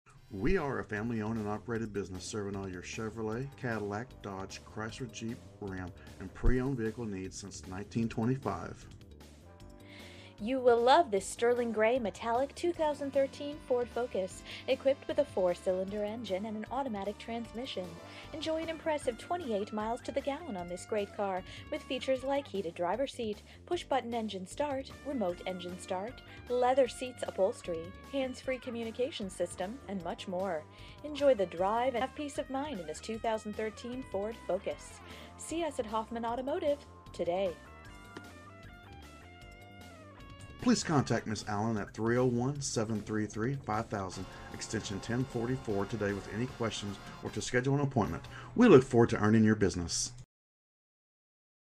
2013 Ford Focus Hoffman Automotive Hagerstown, MD 21740
Ford Focus Imagine driving this Sterling Gray Metallic Ford Focus Sedan, equipped with a 4 Cylinder engine and an automatic transmission.
MILES: 27,894
VIN:1FADP3J27DL384139

Hoffman Automotive
101 South Edgewood Drive
Hagerstown, Maryland 21740